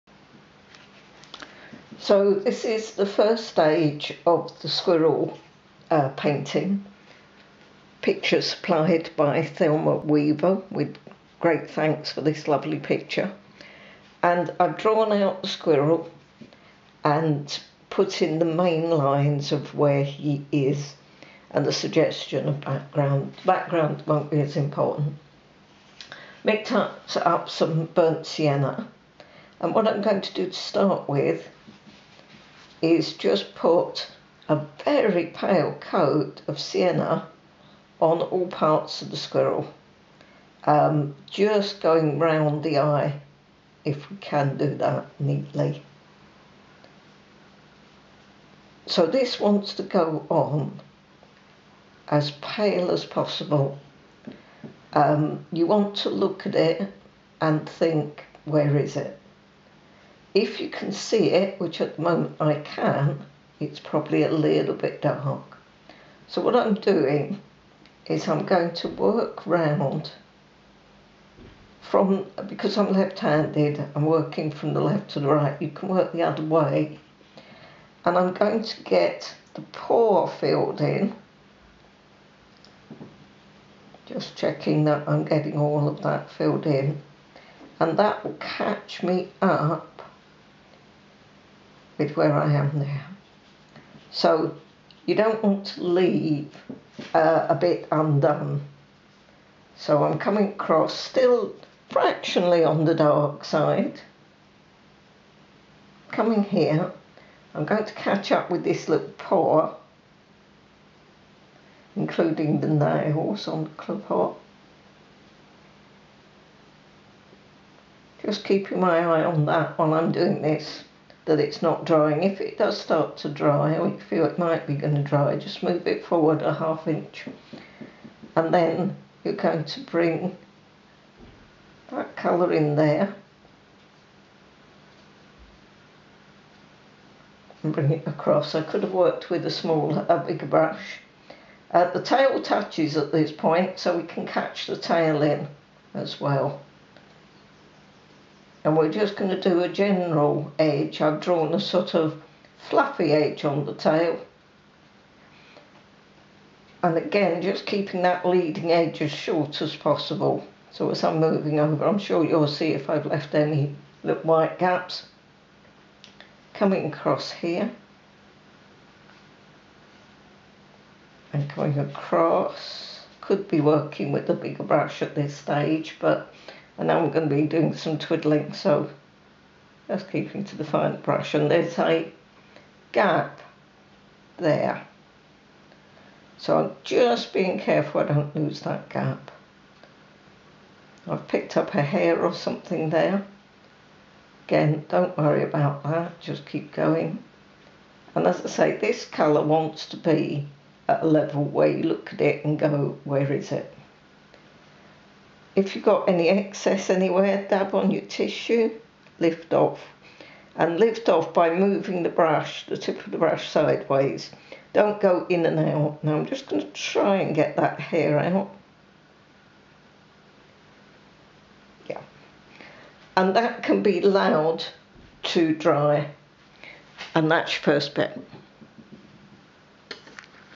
Red Squirrel watercolour part 1
Red squirrel watercolour exercise for learners in isolation.  With thanks to Thelma Weaver for allowing use of the photo.  Stage by stage instructions, part 1 of 10.  By Tina Martin